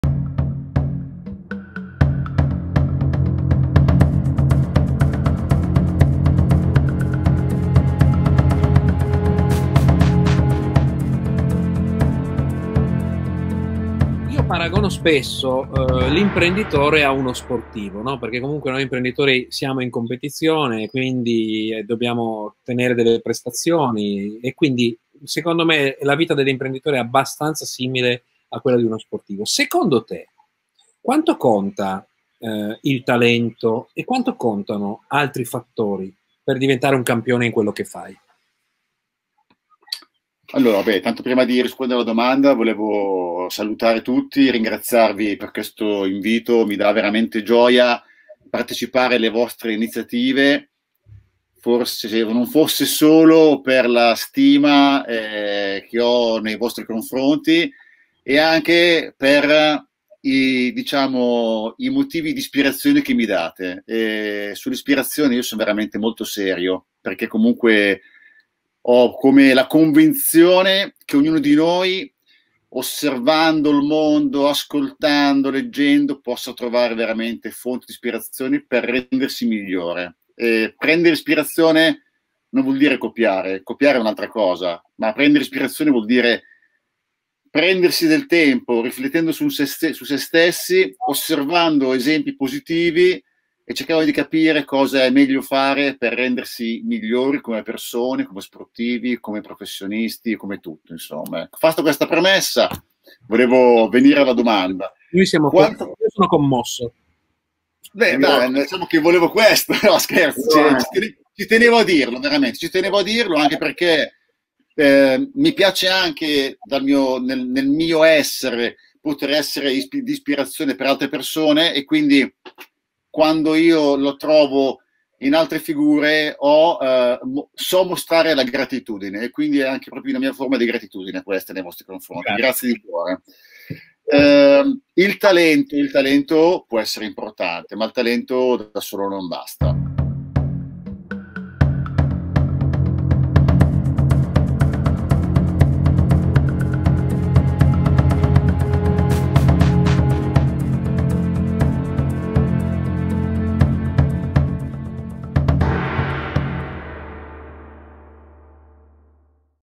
Ispiriamo Andrea Re - Palestra d’Impresa
Per la rubrica “7”: Andrea Re, olimpionico, 8 volte campione del mondo di canottaggio, triathleta, IRONMAN, docente universitario, allenatore. Ci Racconta il talento, i sacrifici, le sconfitte, le vittorie e l’analogia che lo sport ha con il mondo dell’impresa.